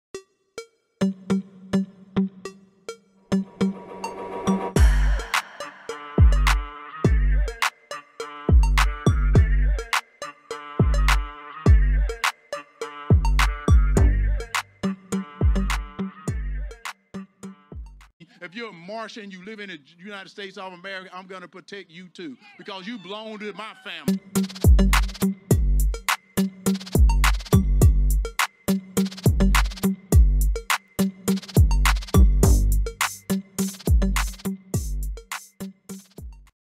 Herschel Walker Says 'Martians' Belong to His Family at Pre Midterm Rally
Herschel Walker, Georgia's Republican candidate for the U.S. Senate, speaks next to Nikki Haley at a campaign event on November 6, 2022 in Hiram, Georgia. Walker said, "If you’re a Martian and you live in the United States of America, I’m gonna protect you, too. Because you belong to my family."